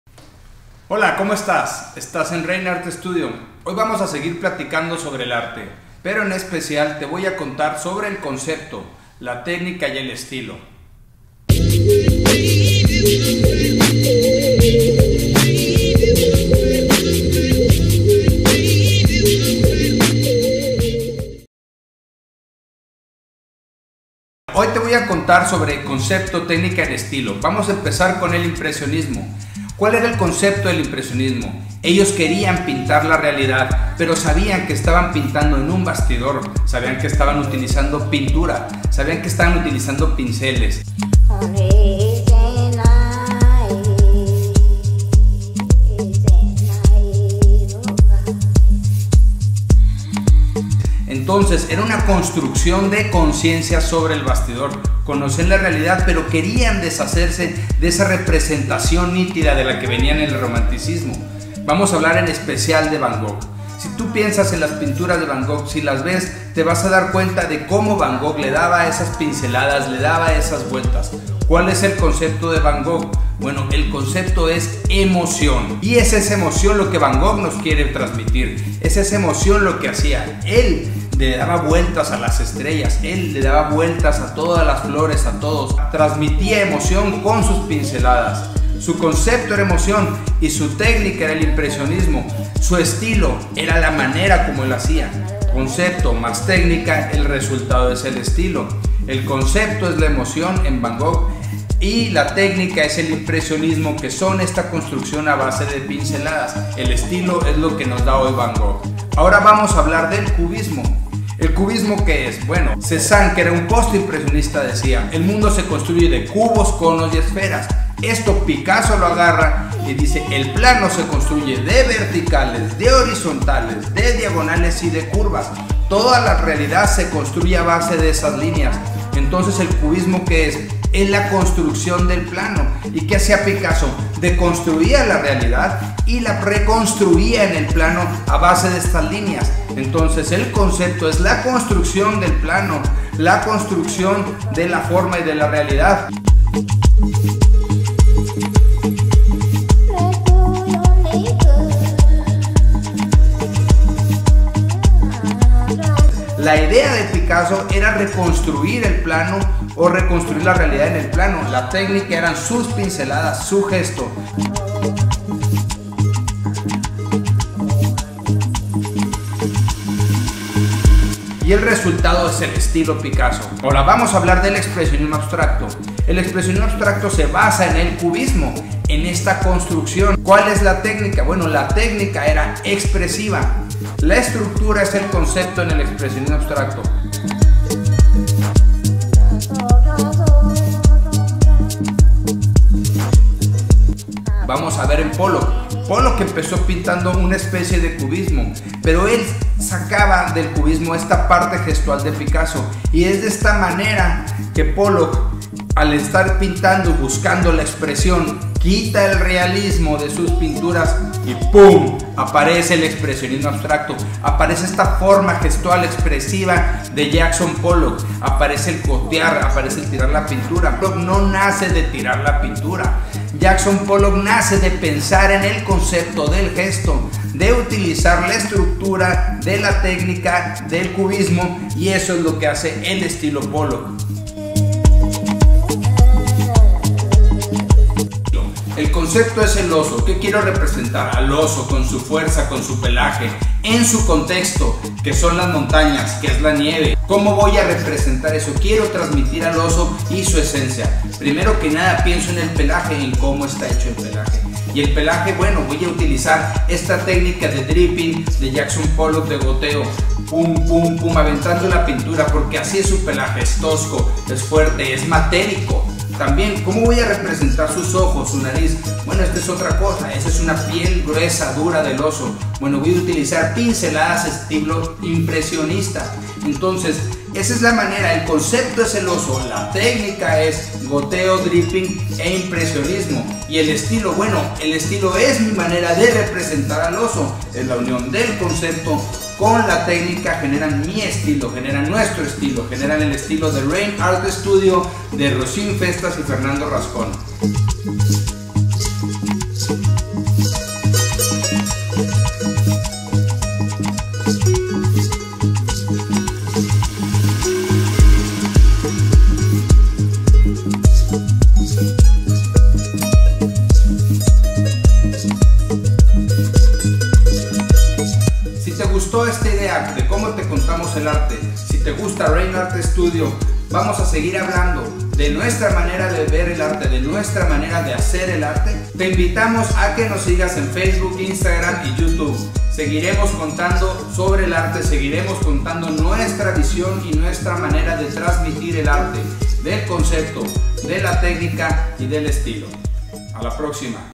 CONCEPTO, TECNICA Y ESTILO
En este capítulo el artista de Rainartstudio Fernando Rascón, nos habla de su visión del concepto, la técnica y el estilo en el arte.